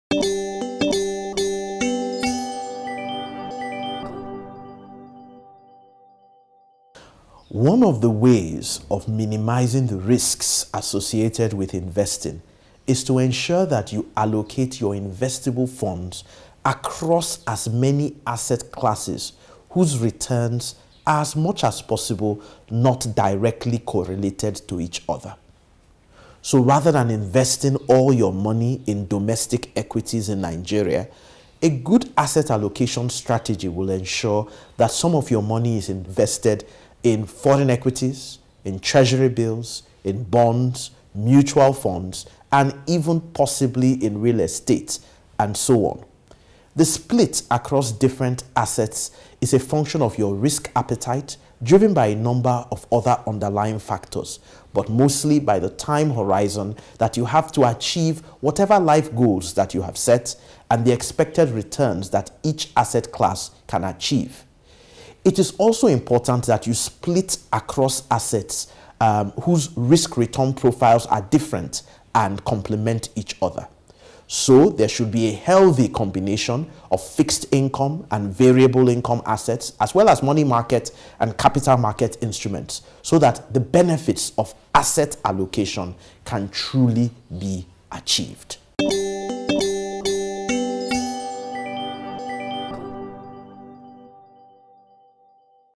Asset Allocation
Asset Allocation helps to reduce the risks of investing. Watch this short clip from Omagbitse barrow to understand what Asset allocation is and how to use it?